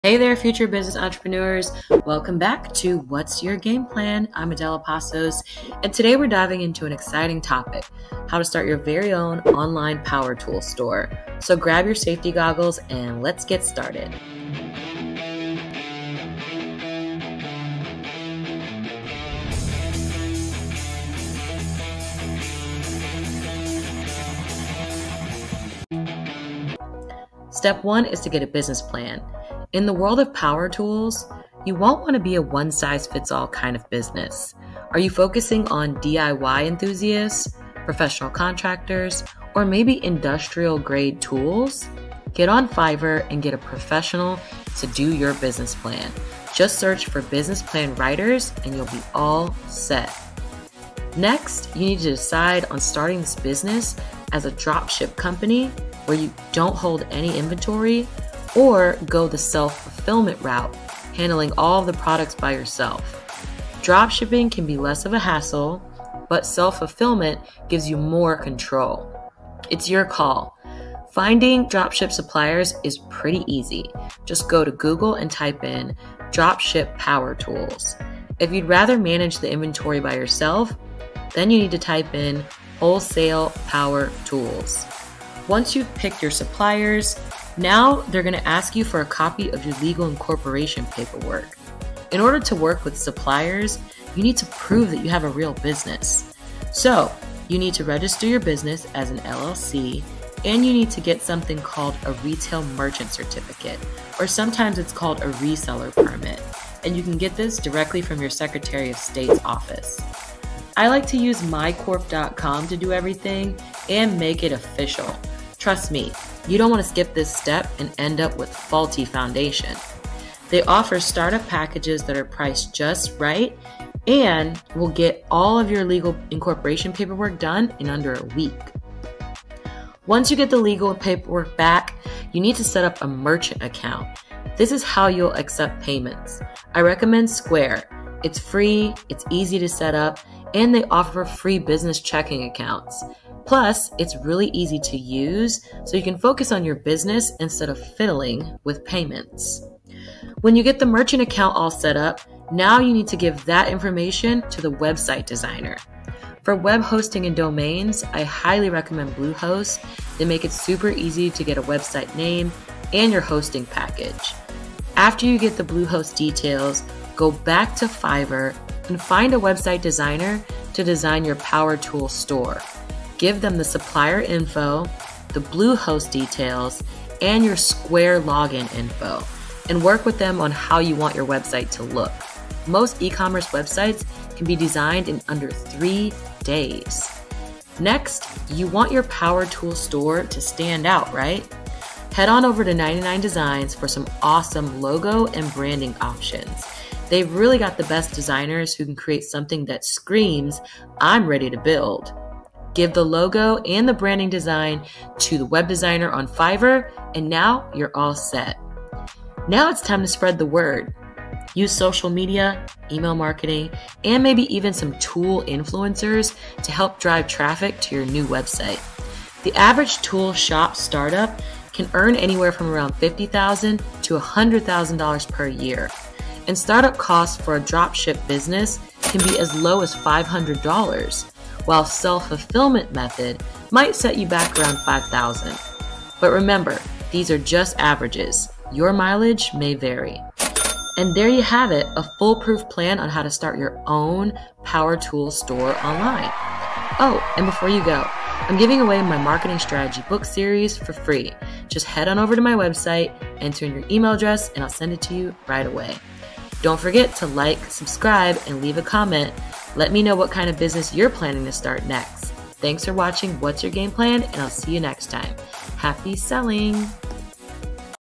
How to Start a Power Tools Business Online 2024
Learn how easy it is to start a power tools business online in 2024 👇

In this episode, we're giving you a step-by-step guide on how to start your very own power tools store online. From choosing the right products to setting up your website and marketing your business, we've got you covered. Whether you're a DIY enthusiast or just looking for a new business venture, this episode will help you build your way to success!

★ Amazon Authors - I just wrote a new book